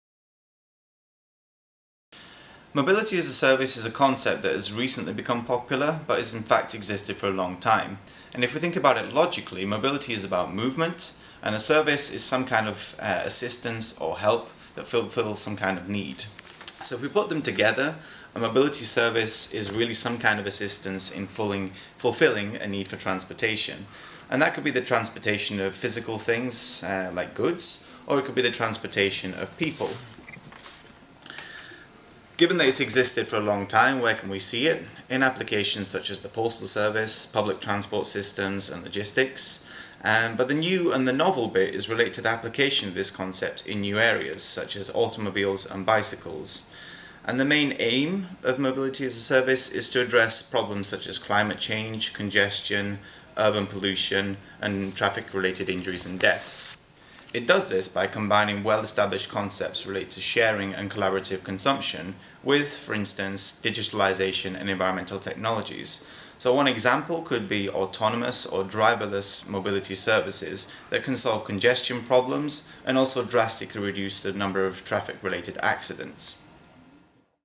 1. Introduction to MaaS
Dr Steven Sarasini introduces Mobility as a Service - MaaS